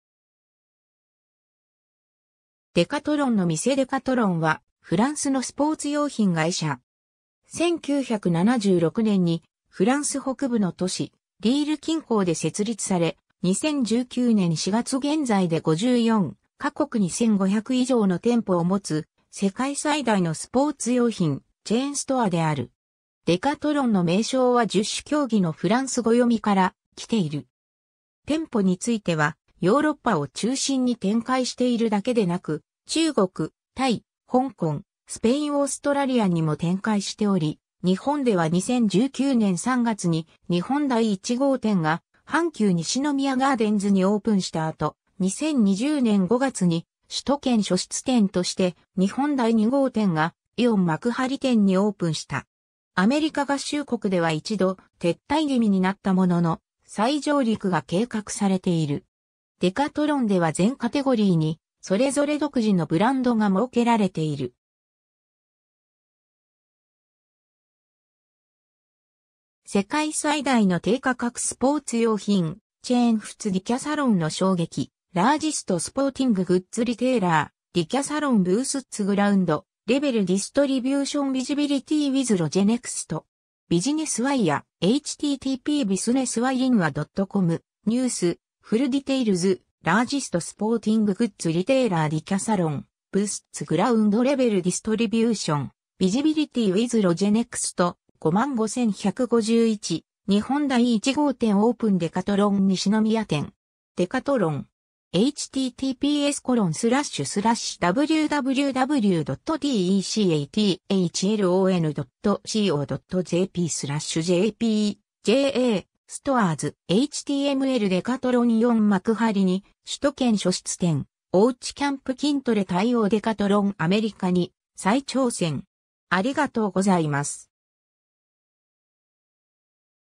デカトロン
デカトロンの店（中国・大連） デカトロン（Decathlon ）は、フランスのスポーツ用品会社。
1976年にフランス北部の都市・リール近郊で設立され、2019年4月現在で54か国に1500以上の店舗を持つ、世界最大のスポーツ用品チェーンストアである。
 デカトロンの名称は十種競技のフランス語読みからきている。
 店舗についてはヨーロッパを中心に展開しているだけでなく、中国・タイ・香港・スペイン・オーストラリアにも展開しており、日本では2019年3月に日本第1号店が阪急西宮ガーデンズにオープンした後、2020年5月に首都圏初出店として日本第2号店がイオン幕張店にオープンした。
アメリカ合衆国では一度、撤退気味になったものの、再上陸が計画されている。
 デカトロンでは全カテゴリーにそれぞれ独自のブランドが設けられている。
^ 世界最大の低価格スポーツ用品チェーン 仏DECATHLON（デカトロン）の衝撃 ^ “Largest Sporting Goods Retailer, Decathlon Boosts Ground-Level Distribution Visibility with LogiNext”. Business Wire. (2017年9月28日). ^ “日本第1号店オープン デカトロン西宮店”. デカトロン. (2019年3月29日). ^ デカトロンイオン幕張に首都圏初出店「おうちキャンプ・筋トレ」対応 ^ デカトロン アメリカに再挑戦